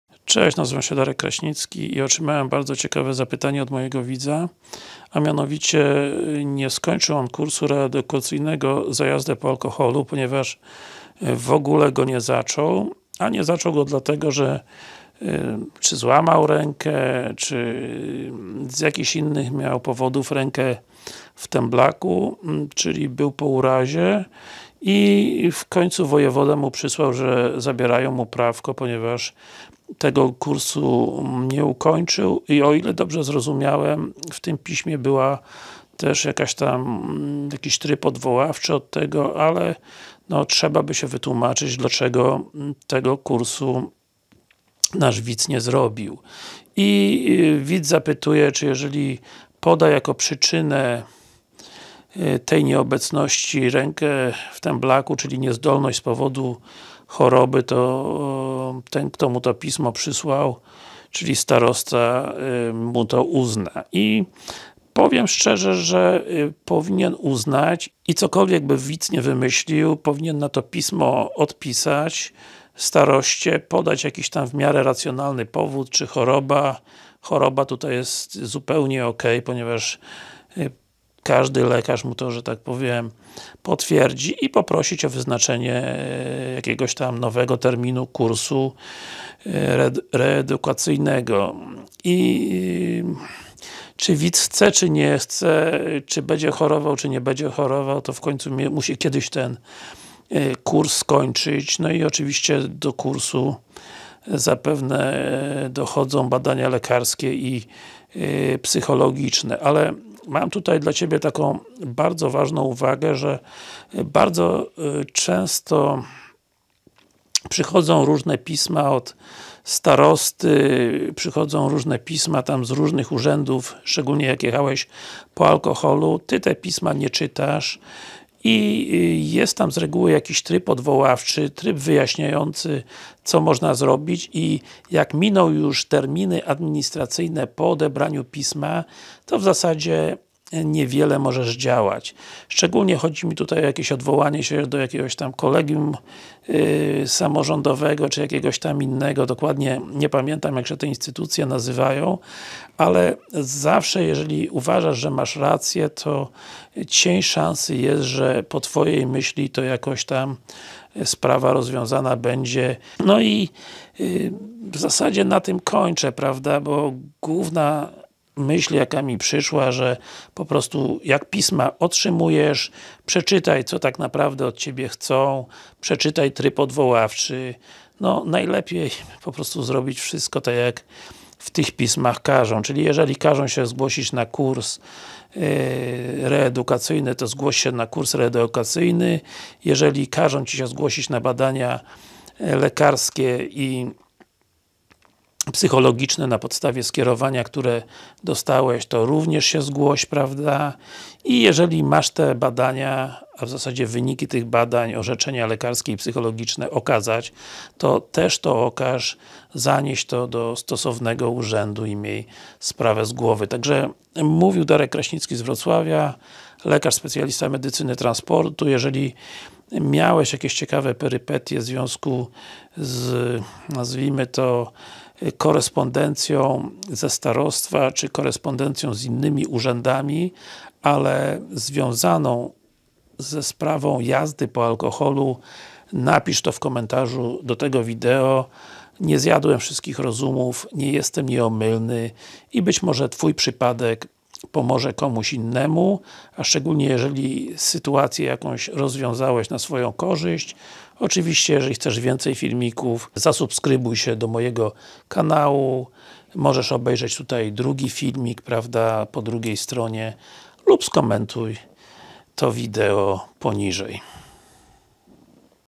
Nie poszedłem na kurs reedukacyjny za alkohol z powodu choroby.
Co robić, jak nie mogłeś pójść na kurs reedukacyjny za jazdę po alkoholu z powodu choroby i otrzymałeś pismo od starosty zabierające Ci prawo jazdy?